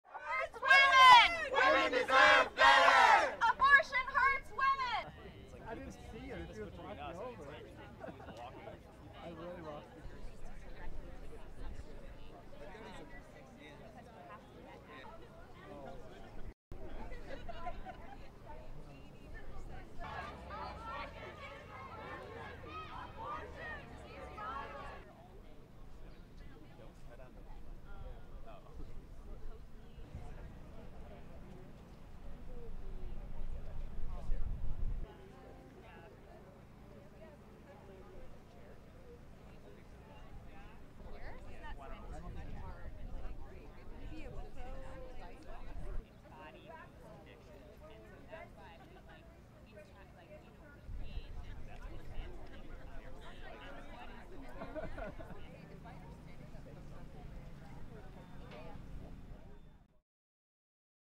People gather outside US Supreme Court after abortion draft ruling leaks | AFP
Abortion rights supporters and anti-abortion rights proponents gather outside the US Supreme Court in Washington, DC the morning after a stunning leak of a draft opinion that signals the institution may be set to strike down the right to abortion.